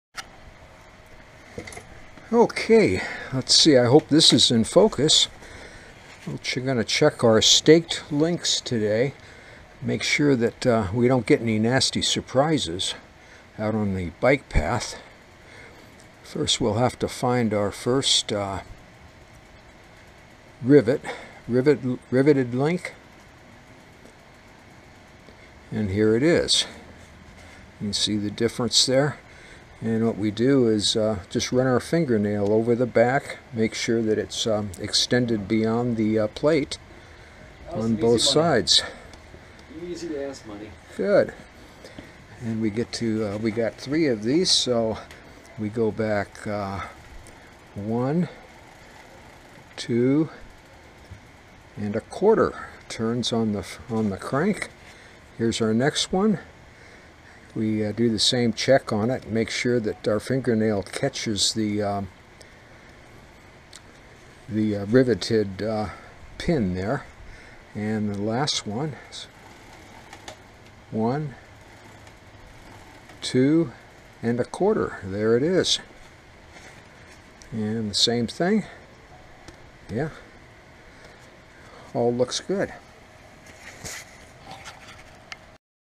Chain rivet pin check X 3 Low Racer Maintenance
Pin type master links are giving way to 'quick links'.
Interestingly motorcycle chains went in the exact opposite direction.
Some folks are not confident using the pin type.
Here's a way to make sure they're installed properly the first time, and check them periodically to make sure they're secure.